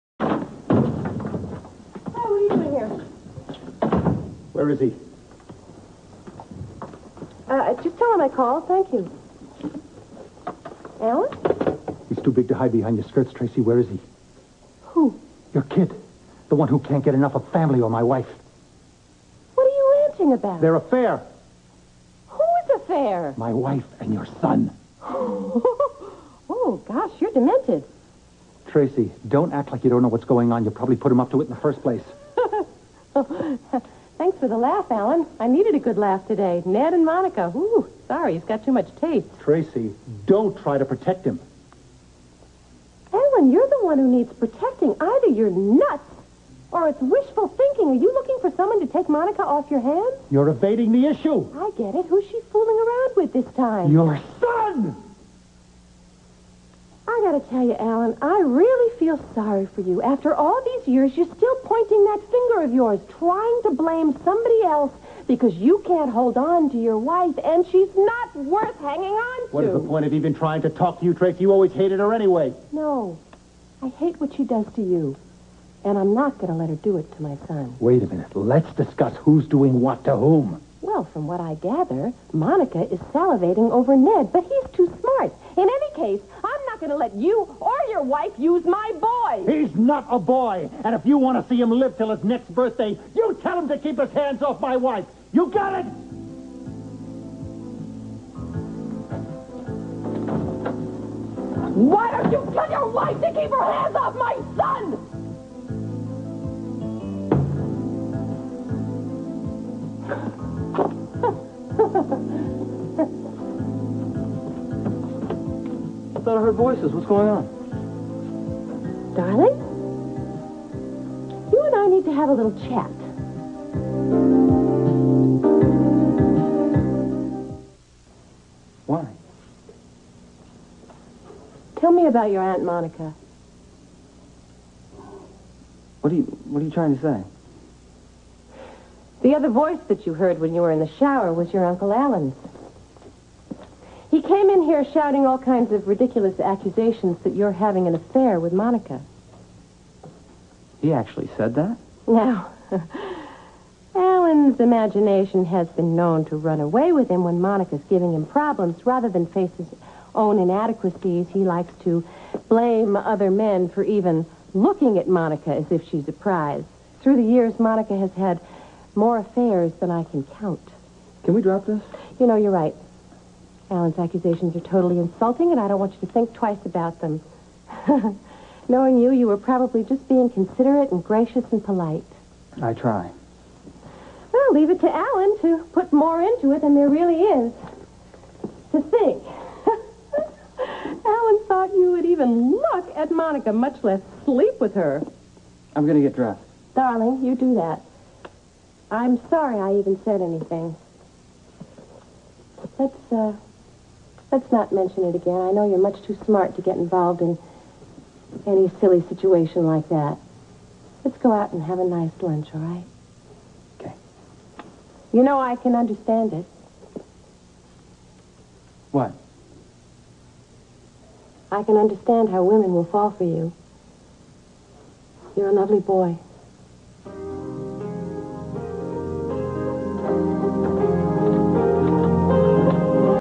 Alan and Tracy argue about Ned and Monica
Alan comes to the gatehouse to confront Ned and finds Tracy instead. He tells Tracy that Ned and Monica are having an affair but Tracy laughs him off. After Alan leaves, Tracy asks Ned, who has been taking a shower, what is going on with Monica. Ned hedges.

(edited from clips by lovegymnasts)